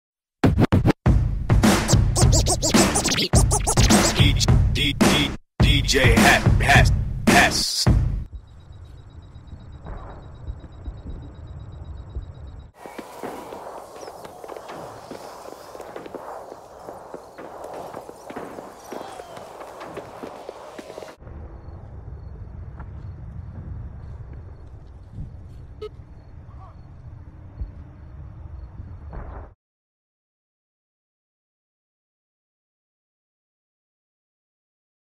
Is Call of Duty Elite Worth the money?
i would say... no not at this time lol